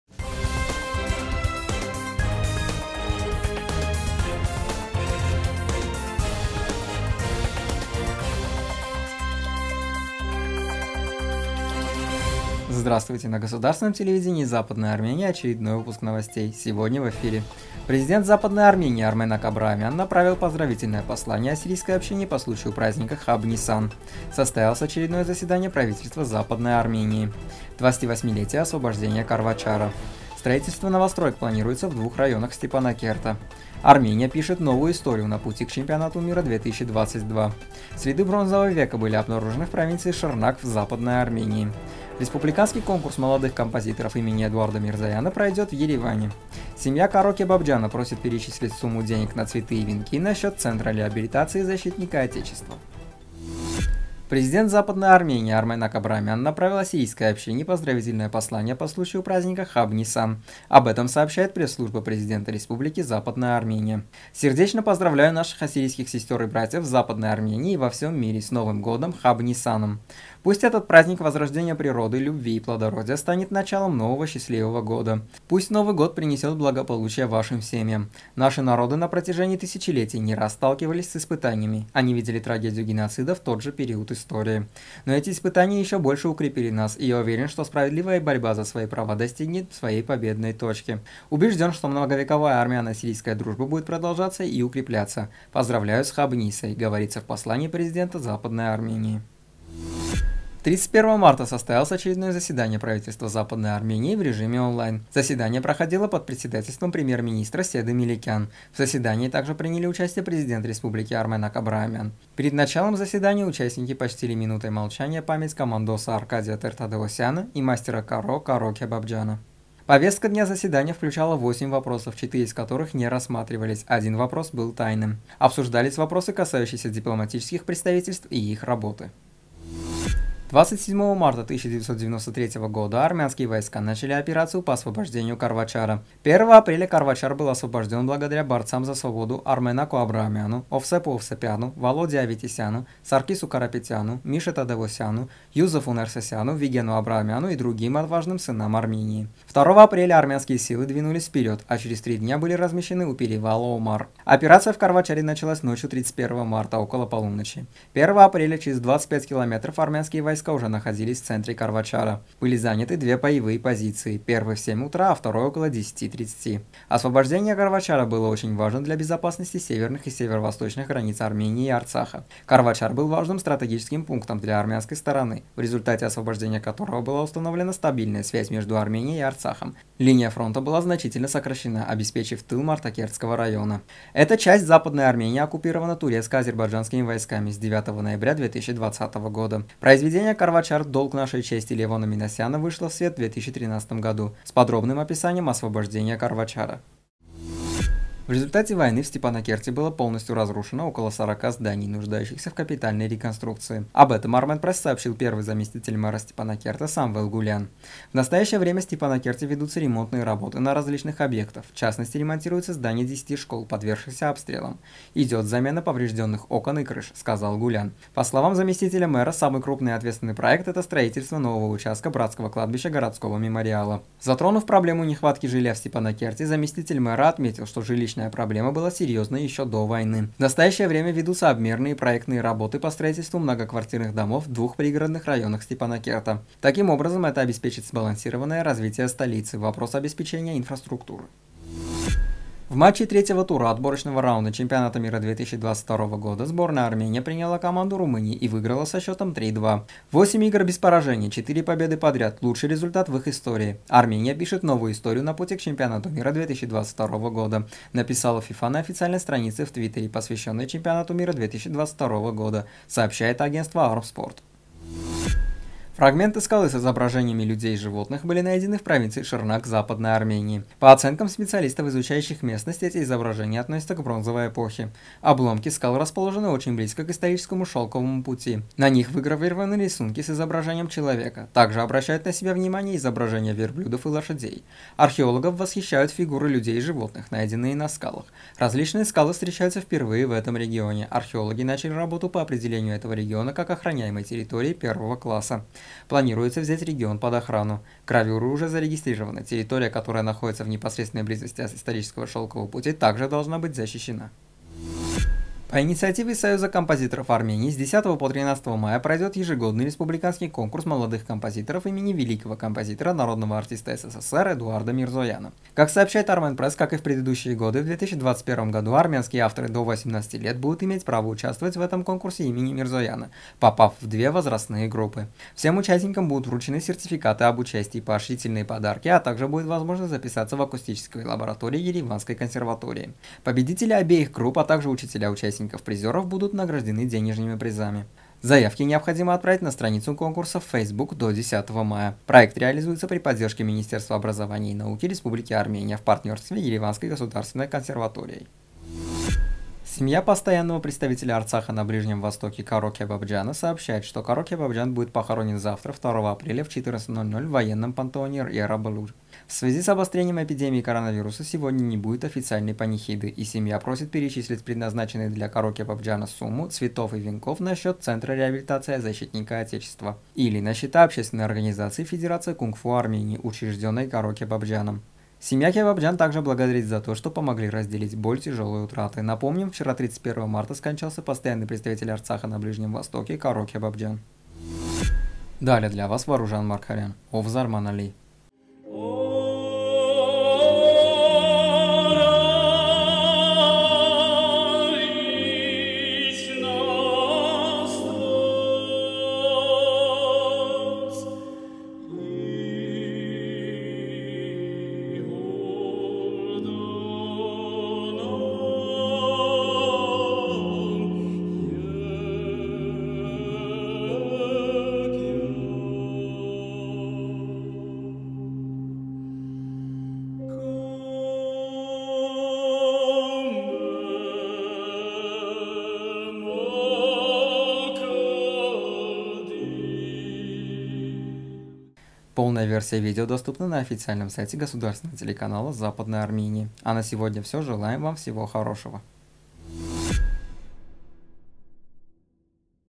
28и-летие освобождения Карвачара.Новости 01.04.2021
1.Президент Западной Армении Арменак Абраамян направил поздравительное послание ассирийской общине по случаю праздника Хаб-Ниссан
2․Состоялось очередное заседание Правительства Западной Армении.
3. 28и-летие освобождения Карвачара
4.Строительство новостроек планируется в двух районах Степанакерта.
5.Армения пишет новую историю на пути к ЧМ-2022․
6.Следы бронзового века были обнаружены в провинции Шрнак в Западной Армении.
7.Республиканский конкурс молодых композиторов имени Эдуарда Мирзояна пройдет в Ереване
8․Семья Каро Кабабджяна просит перечислить сумму денег на цветы и венки на счет Центра реабилитации защитника Отечества.